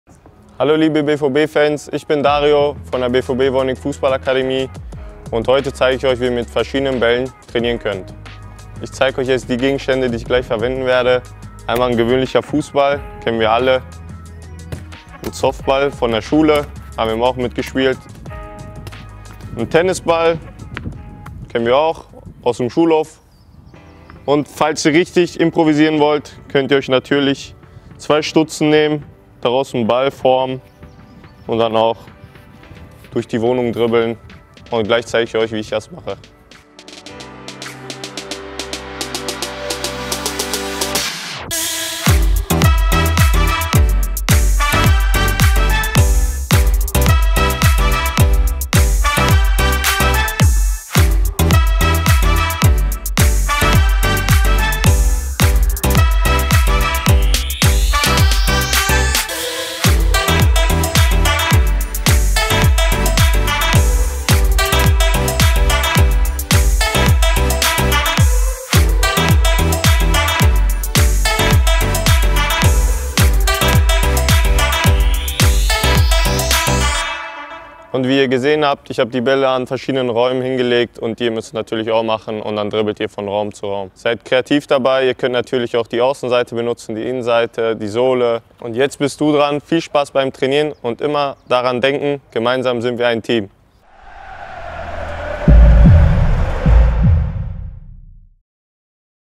Dribbling exercise with Dario Scuderi | soccer training at home | BVB-Heimspielkurs #2
Today our coach Dario will show you how you can improve your dribbling skills at home. If you don't have a ball, no problem! Dario will show you which materials you can easily form a ball from! Warm up yourself and off you go!